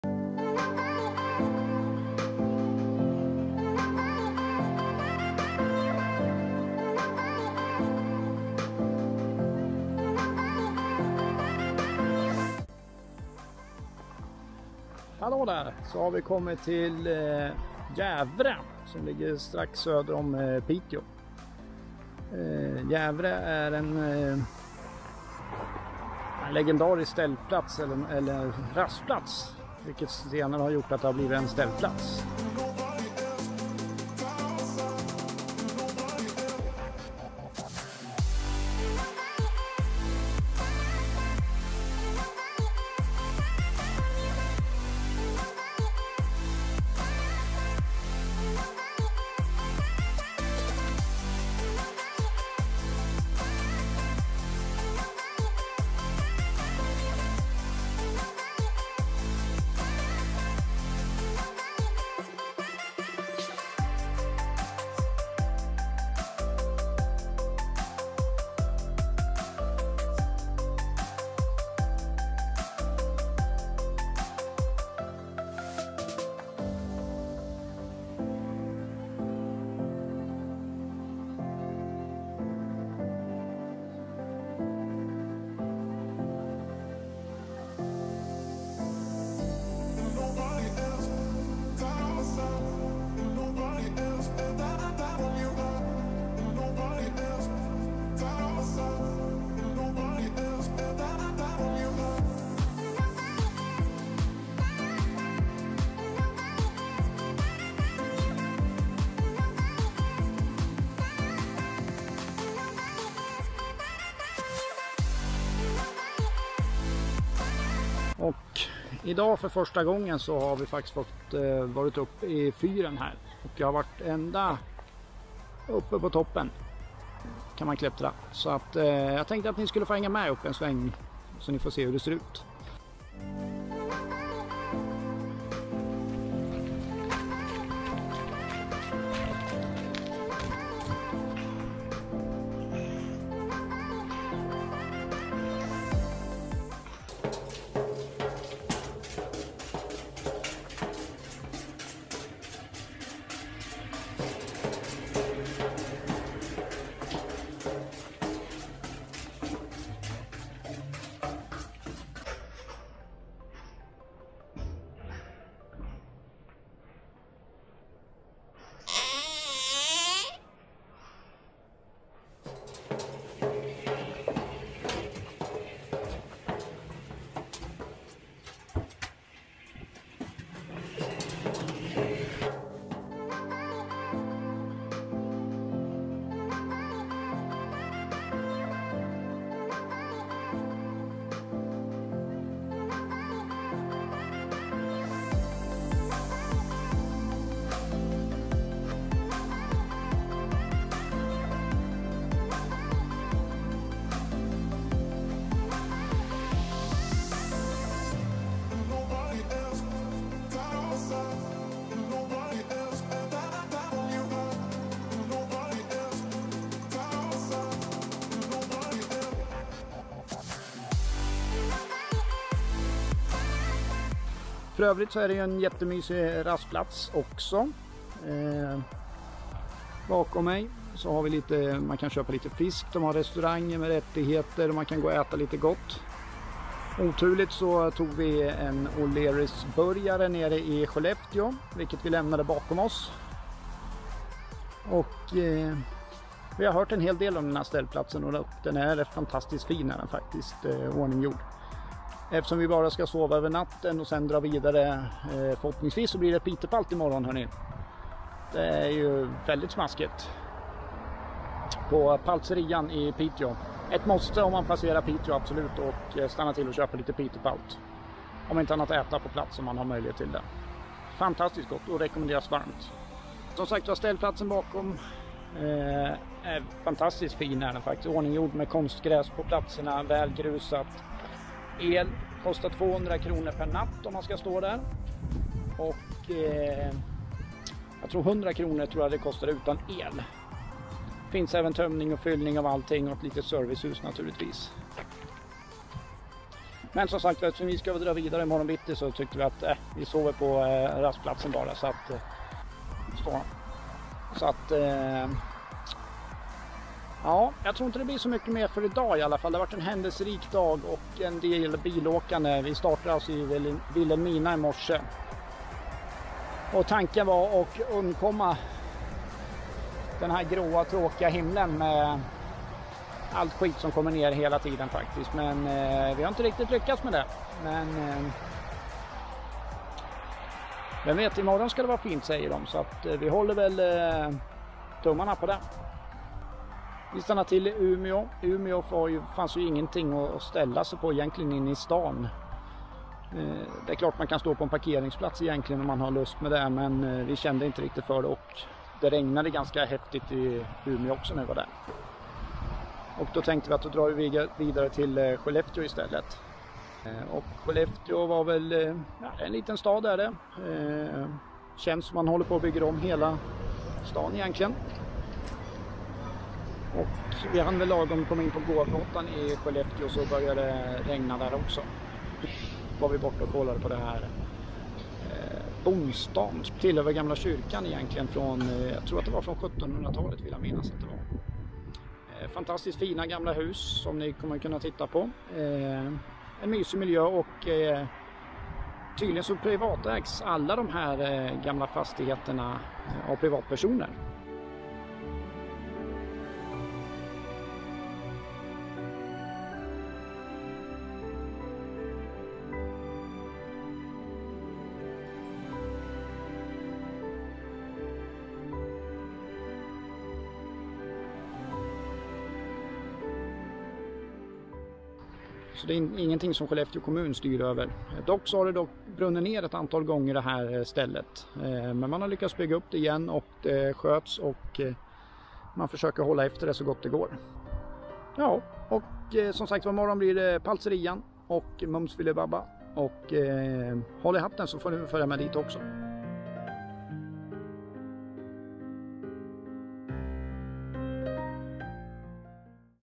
Skellefteå & Jävre rast- & ställplats
En kort film om Skellefteå & Jävre ställplats  sommaren 2020 med vår husbil.
A short movie about Skellefteå & Jävre camperstop in summer 2020 with our RV.